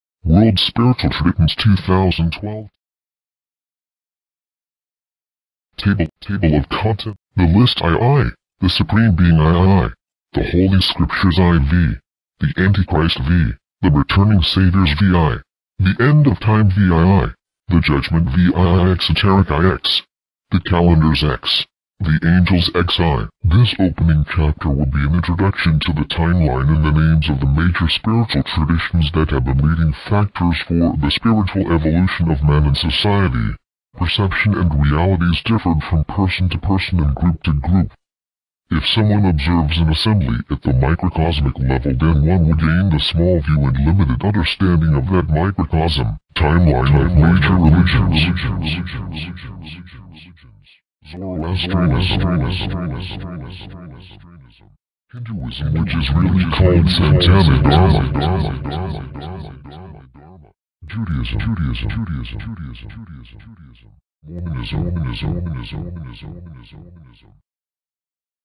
Planet X Intro to World Spiritual Traditions
A timeline and list of major World Spiritual Traditions for then Planet Earth. Channeled from the Spiritual realm of Planet X.